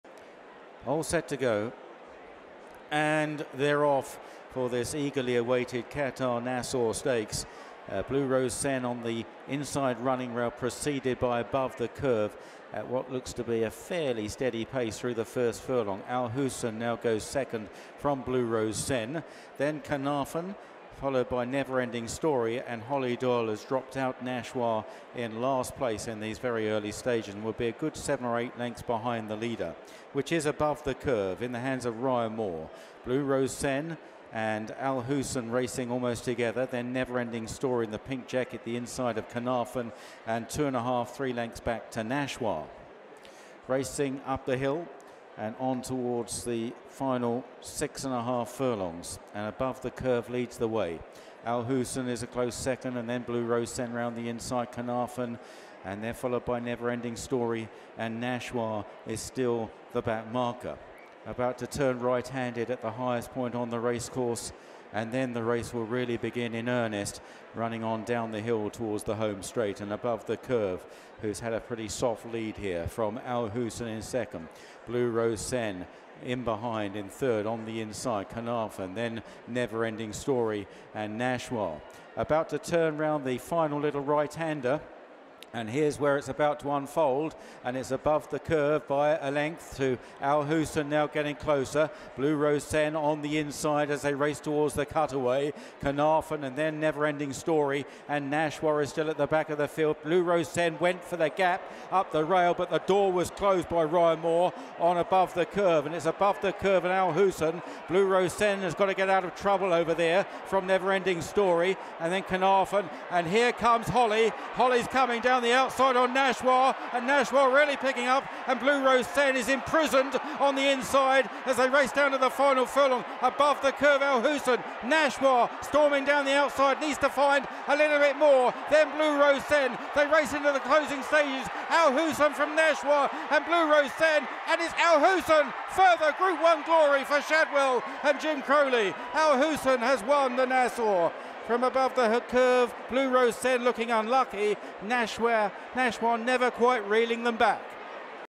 上がり馬アルフスンが金星！仏二冠牝馬ブルーローズセンは4着【ナッソーS 2023】/ Al Husn【Qatar Nassau Stakes 2023】
現地8月3日、イギリスのグッドウッド競馬場でG1ナッソーステークス（3歳以上牝、芝2010m）が行われ、J.クローリー騎乗のアルフスンが格上のG1馬たちを下す金星を挙げた。

レースはアバブザカーブが逃げ、アルフスンは内のブルーローズセンと並ぶように2番手を追走、ナシュワは最後方に待機する。残り約2ハロンでアルフスンが仕掛けてアバブザカーブをかわすと、ナシュワやブルーローズセンの追撃もしのいで先頭でゴールに飛び込んだ。

半馬身差の2着は逃げ粘ったアバブザカーブ。さらに半馬身差の3着にナシュワが続き、1番人気に推されていた仏二冠牝馬ブルーローズセンはさらにクビ差の4着までだった。

R.ヴェリアン厩舎のアルフスンはドバウィ産駒の4歳馬。前走のオールウェザーの英G3ホッピングスフィリーズステークスではナシュワを2着に下しての重賞初制覇を果たしており、今回もナシュワを返り討ちにするとともにG1初勝利を手にした。

英競馬メディア『At The Races』によると、ヴェリアン調教師は次走について仏G1ジャンロマネ賞と英G1ヨークシャーオークスを候補に挙げており、その後は仏G1オペラ賞と米G1ブリーダーズカップフィリー＆メアターフへの出走を示唆しているという。